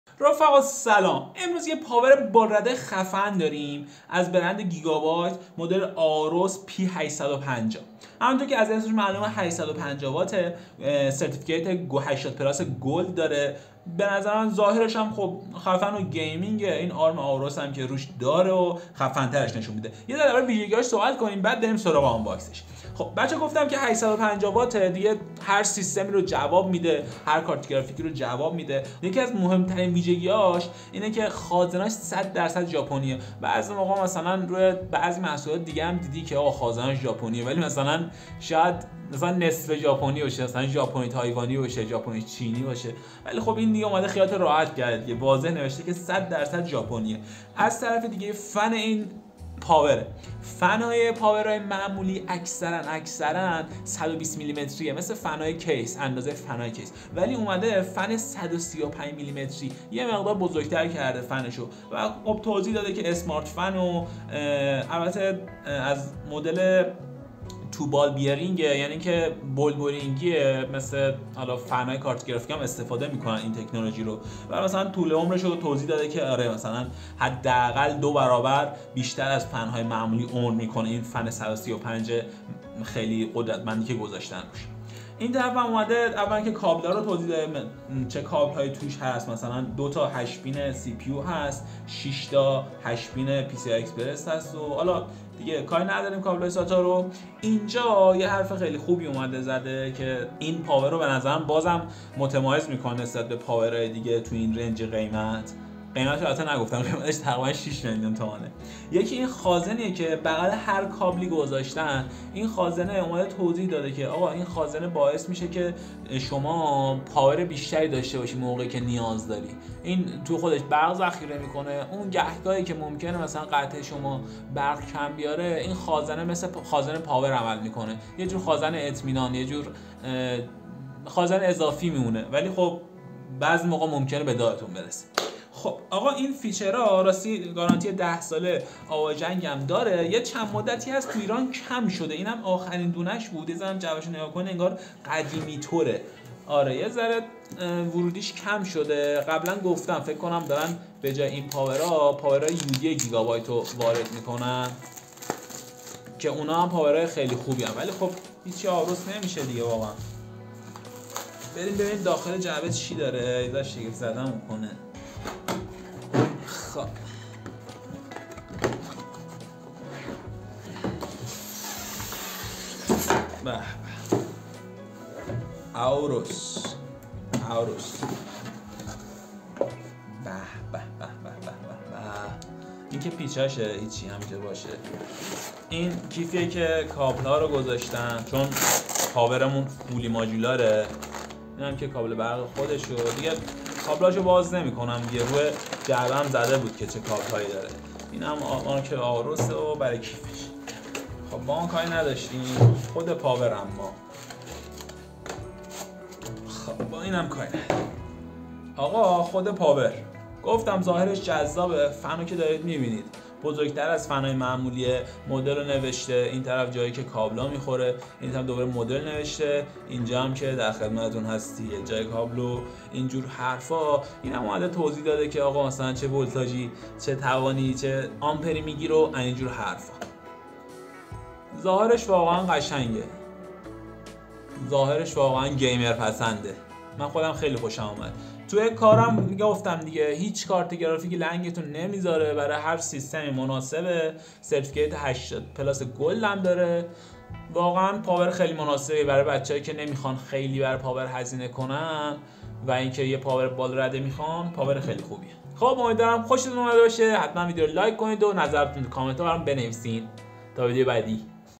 آنباکس و بررسی پاور گیمینگ AORUS 850W | تضمین ده ساله
آنباکس و بررسی پاور گیمینگ AORUS 850W | انتخاب ارزان و به صرفه برای سیستم های بالا رده

به نظرتون فرق زیادی ایجاد شده نظرتون تو کامنت برام بنویسید
برای دوستانتون به اشتراک بزارید.

همین طور از طریق لینک ها زیر می توانید با بنده در ارتباط باشید.